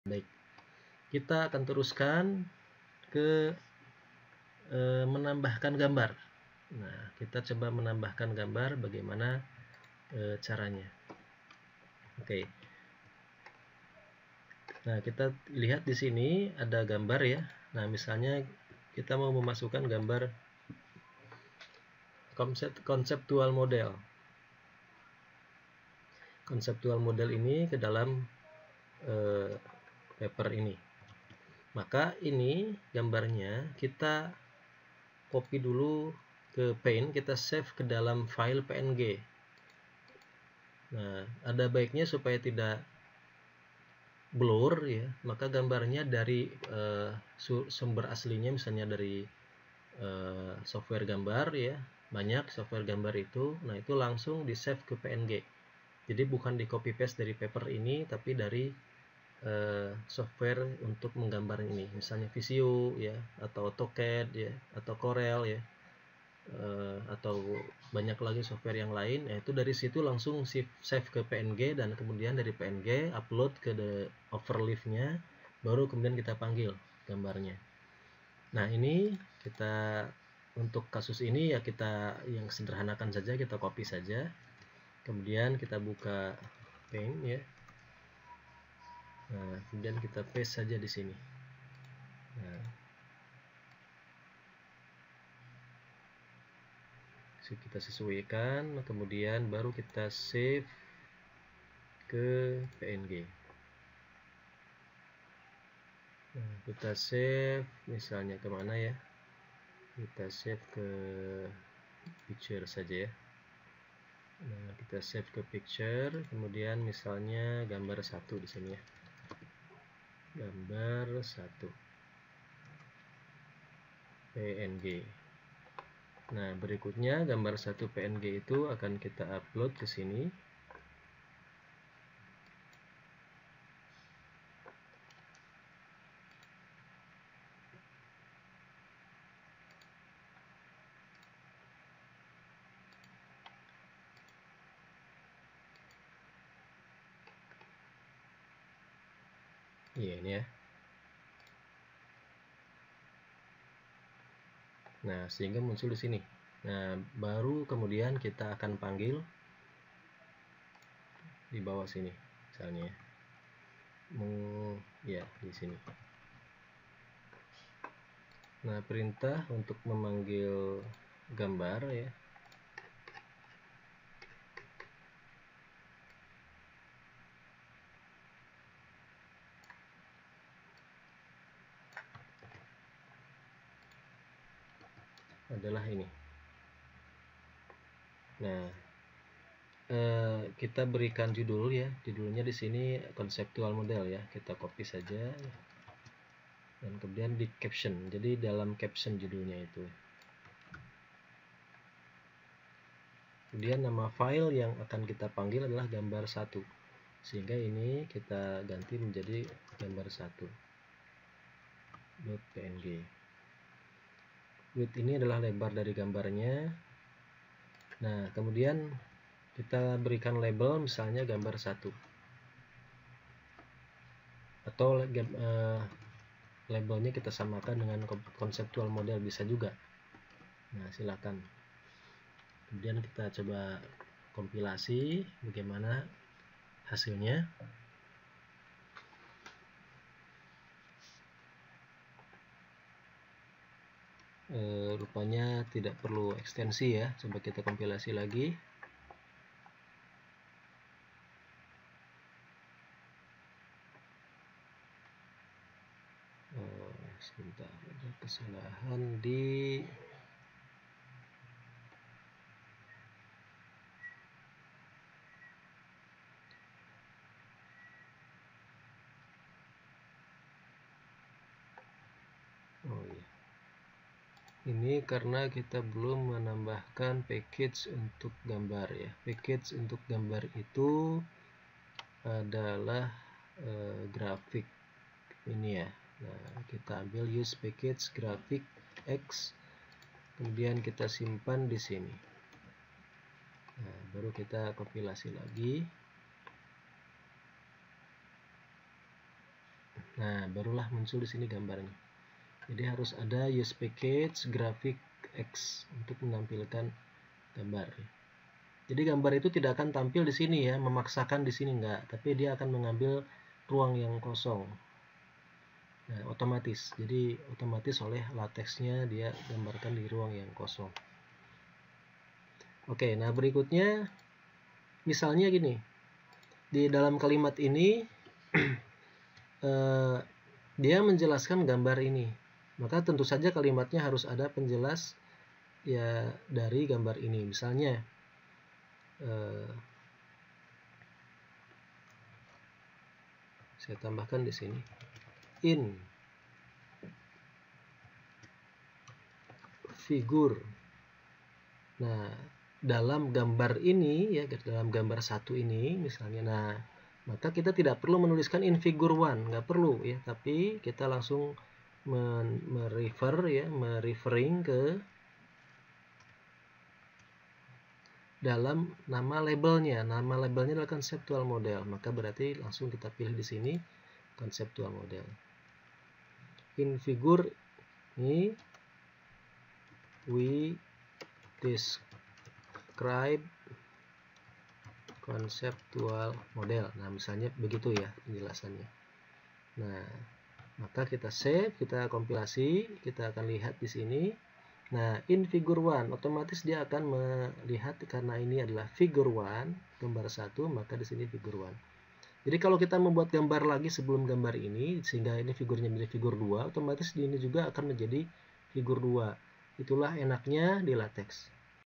Latex pada Overleaf - Gambar / Figure (Bagian 3)
Part 3 Latex with Overleaf:

Uploading image
Search for and use package from figures
Calling pictures from latex

Part 3 Latex dengan Overleaf  :

Mengunggah gambar
Mencari dan menggunakan package dari figure
Memanggil gambar dari latex